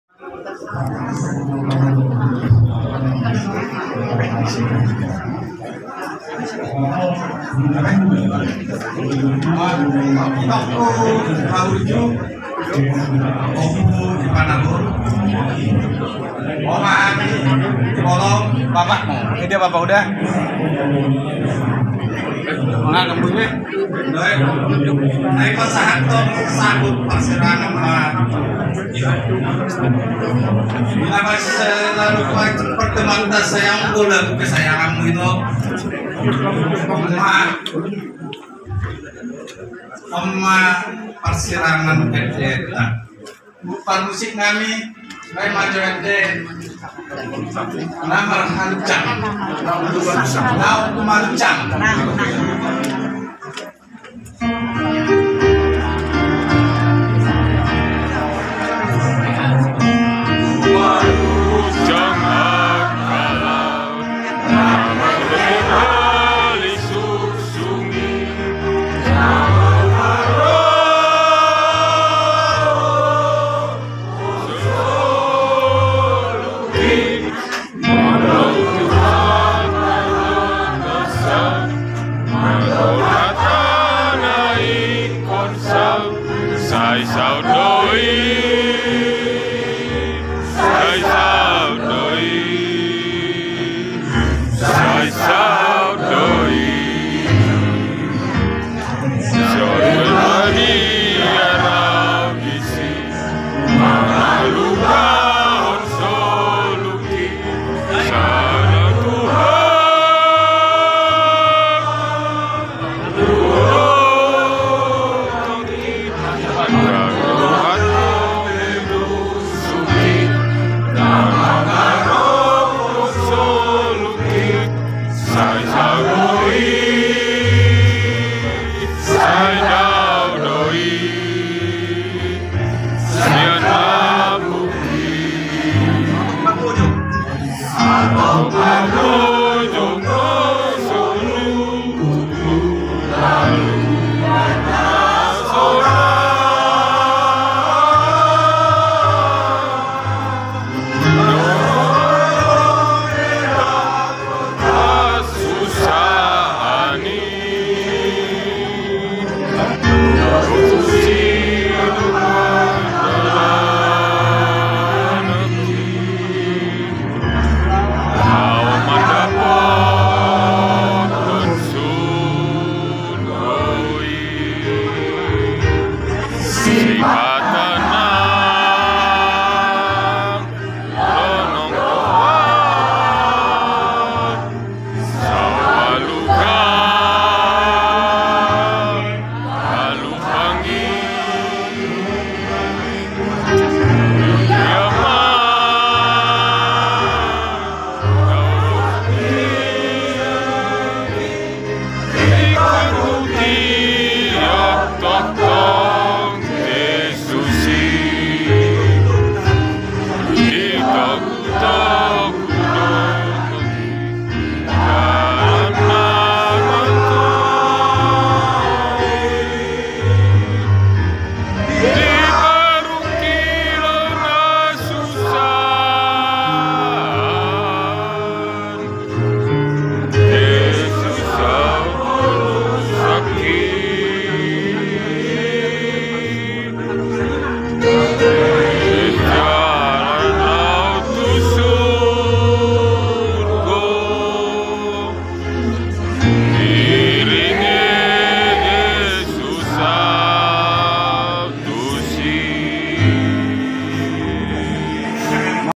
Acara Adat di Partua ni Ny Sigalingging br Sitorus (Op Ivanna) -13
Rumah Duka RS UKI, 29 Oktober 2023